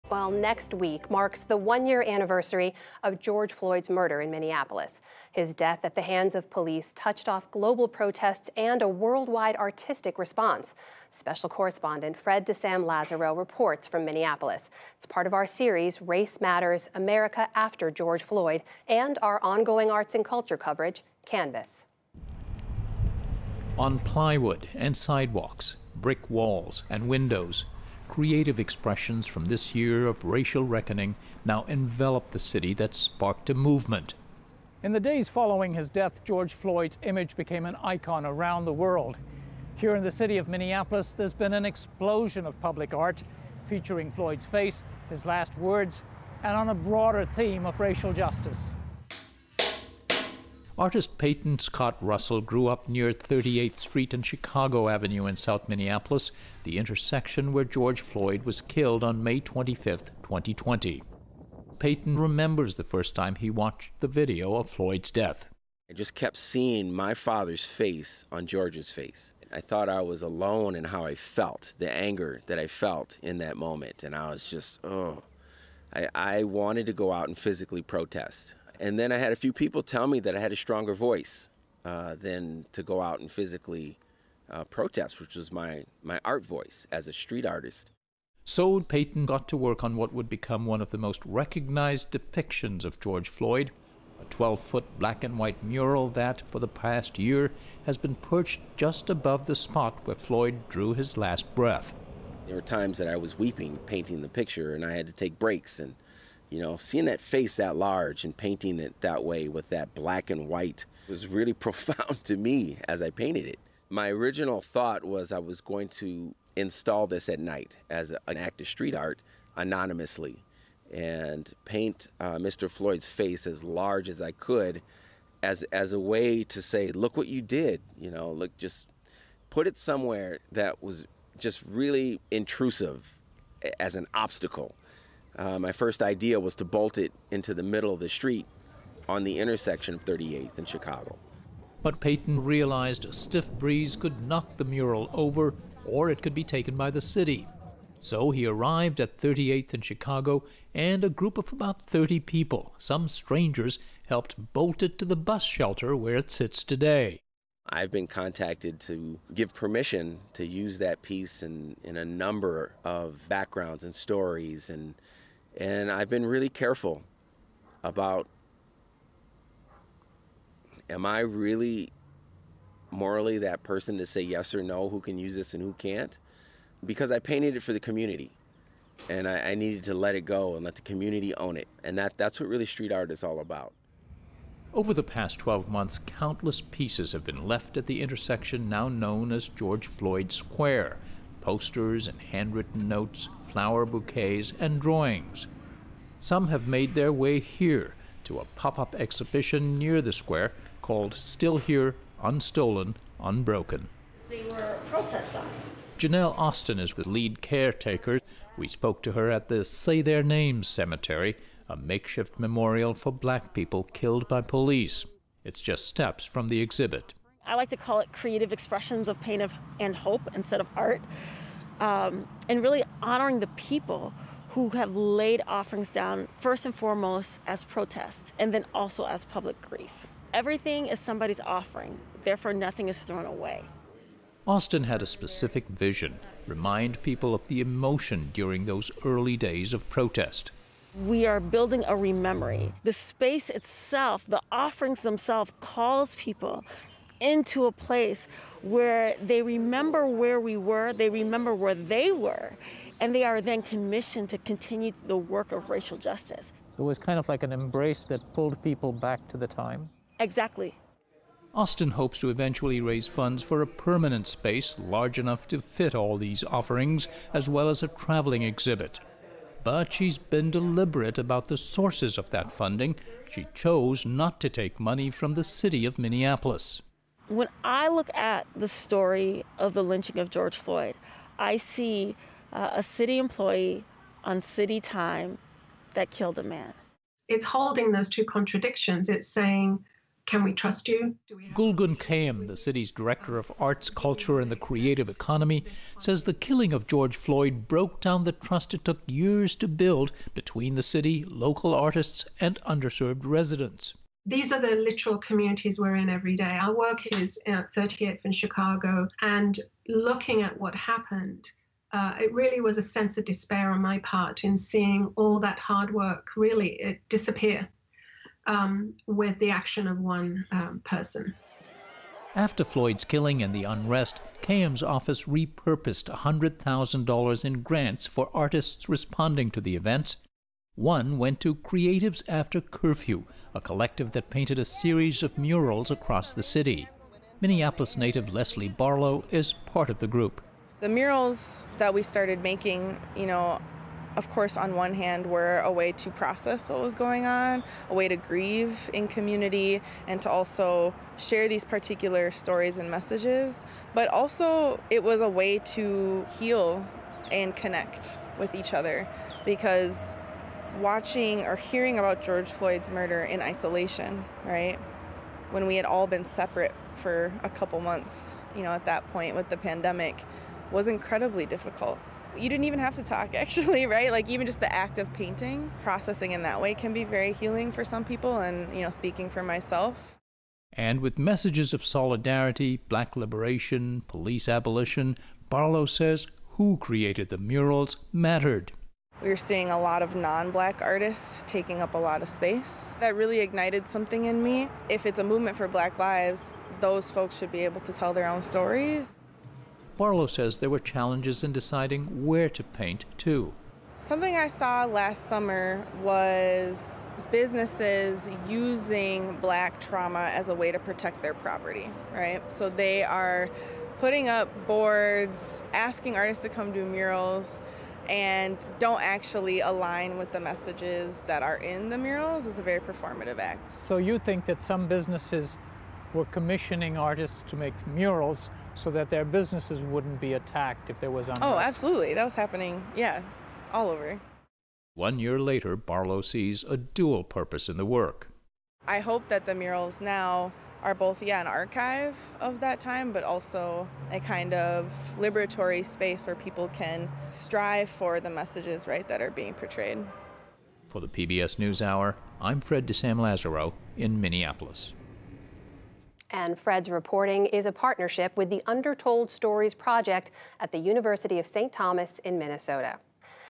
How George Floyd's image became an icon for artists and helped communities mourn
Next week marks the anniversary of George Floyd's killing in Minneapolis. His death at the hands of police touched off global protests and a worldwide artistic response. Special correspondent Fred de Sam Lazaro reports from Minneapolis for our series, "Race matters: America After George Floyd," and our ongoing arts and culture coverage, CANVAS.